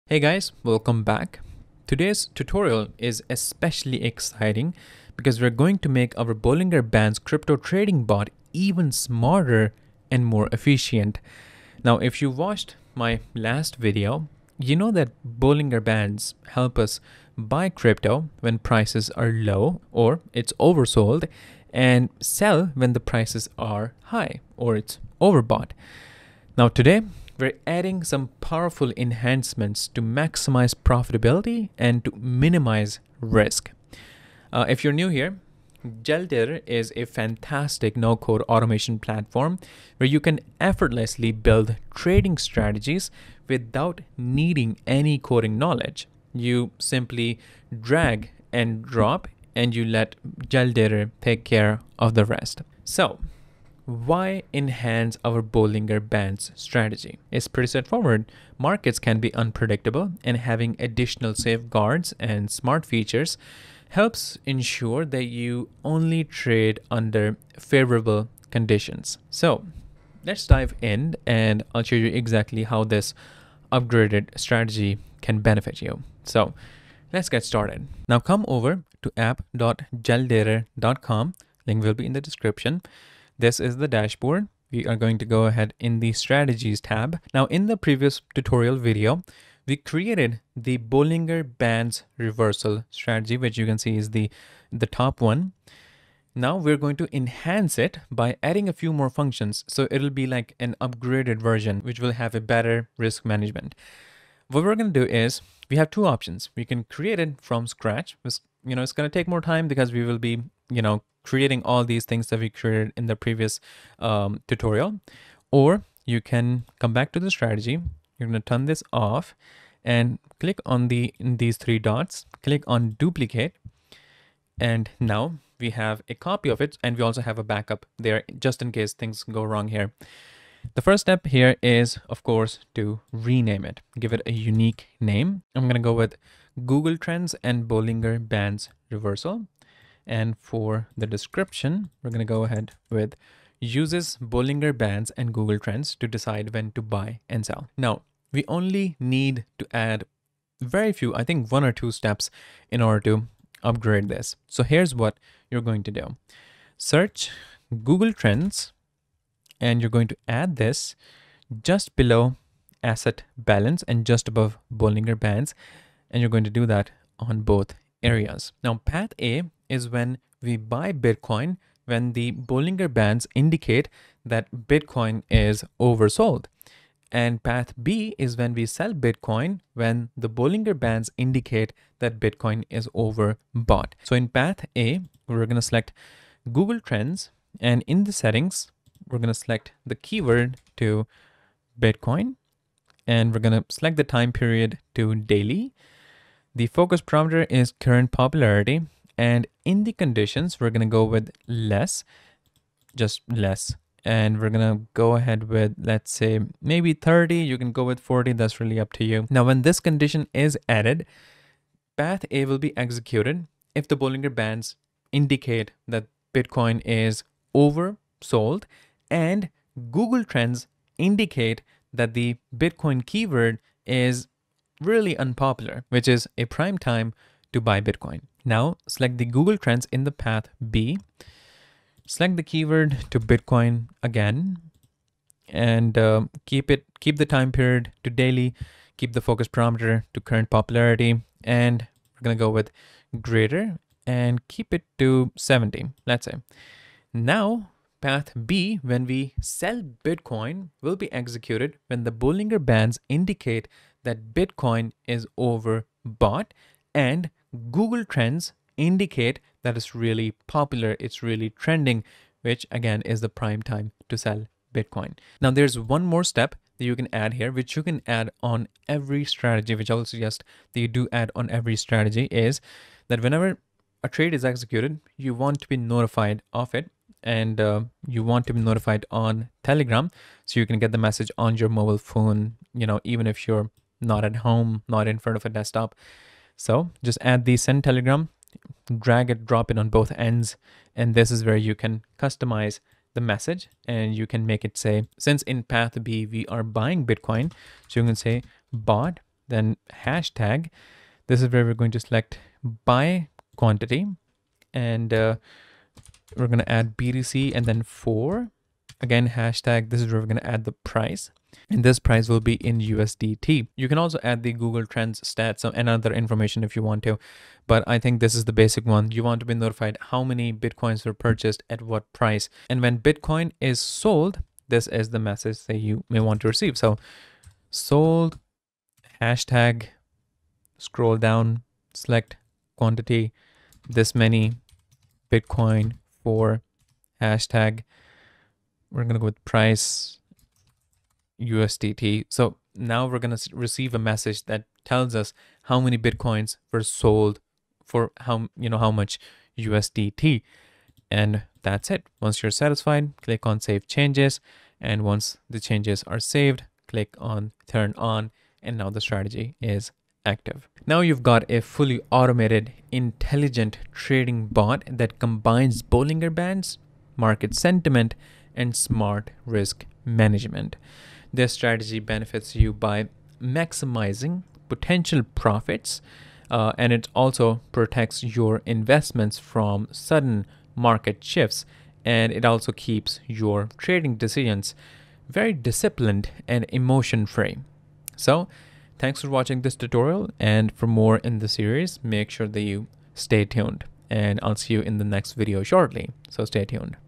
Smarter Bollinger Bands Bot: Adding Google Trends & Telegram Alerts | Jellydator Tutorial #5
In this Jellydator tutorial, we upgrade our Bollinger Bands bot by integrating Google Trends metrics and Telegram alerts.